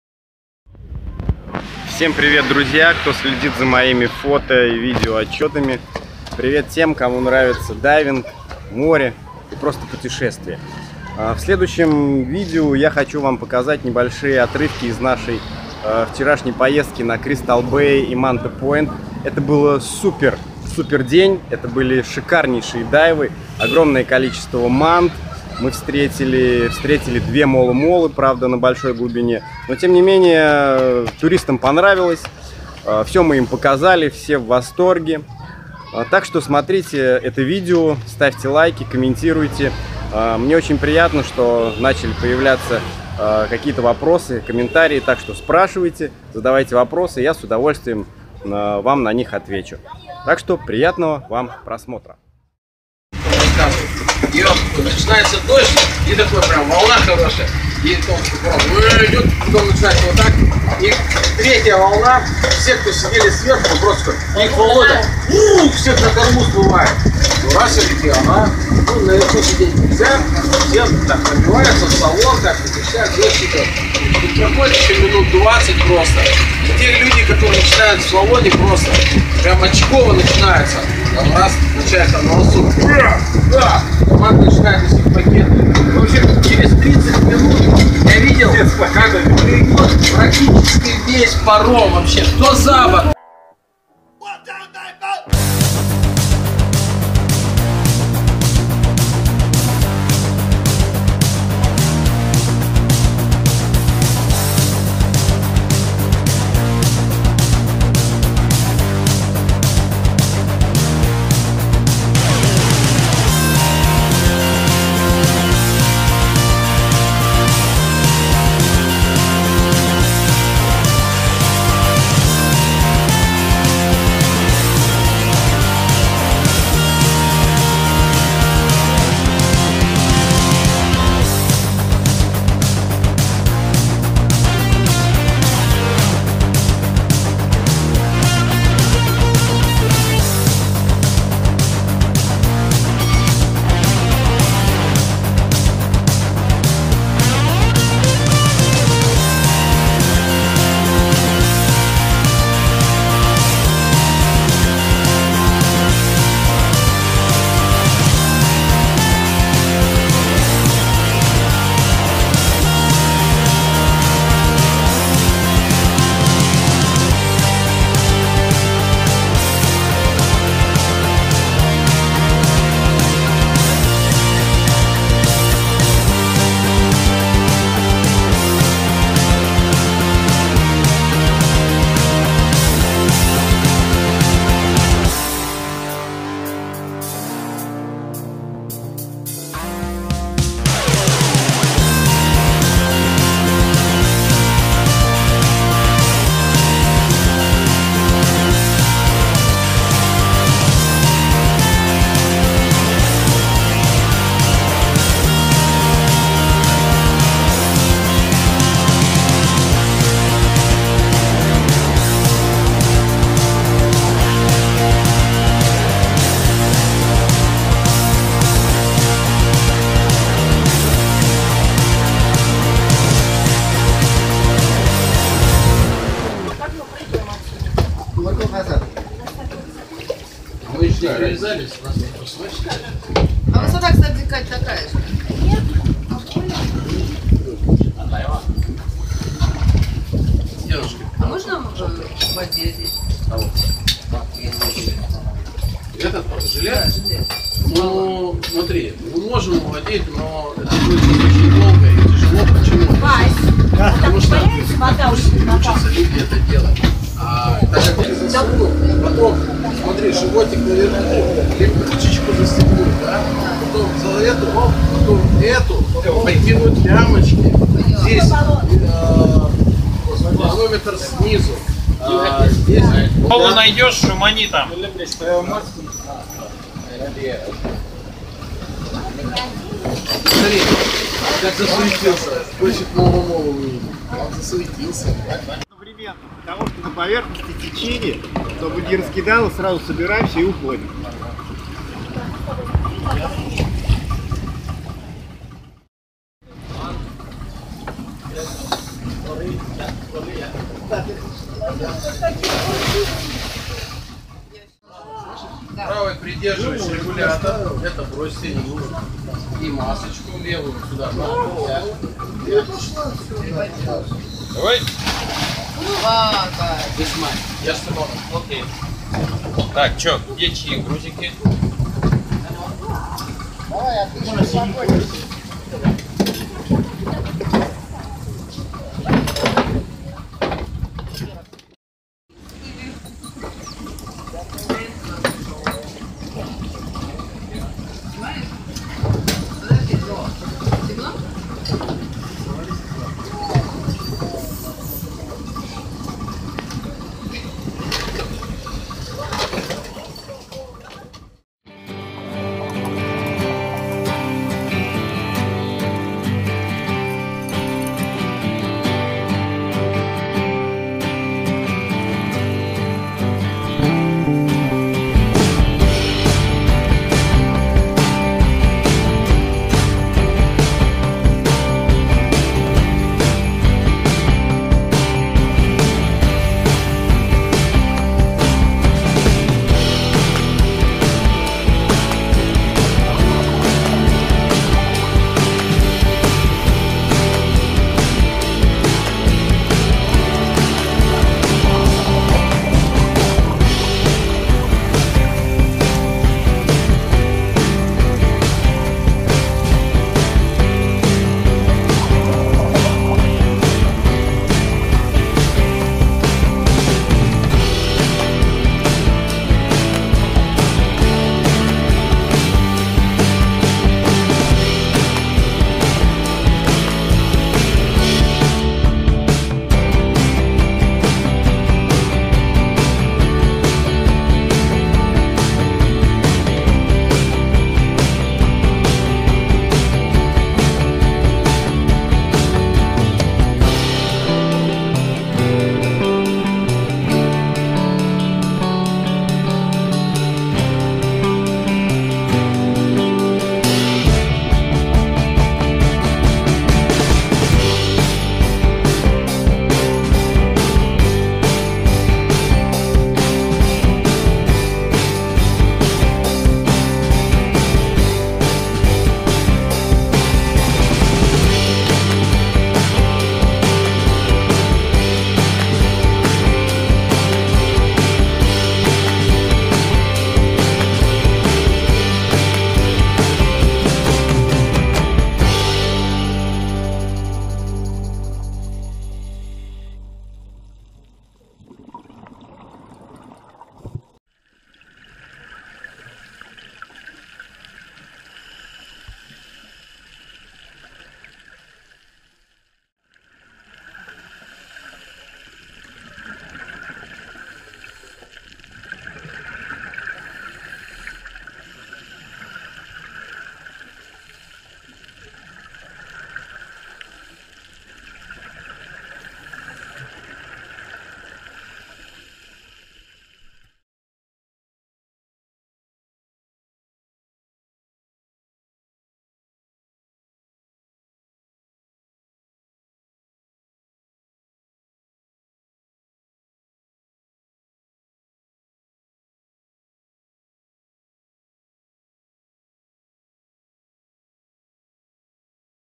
Дайвинг на Бали. CrystalBay and MantaPoint
Шикарные дайвсайты на острове Нусапенида. CrystalBay and MantaPoint. Грациозные манты, таинственная Мола мола. Кристально чистая вода. Дайвинг на Бали.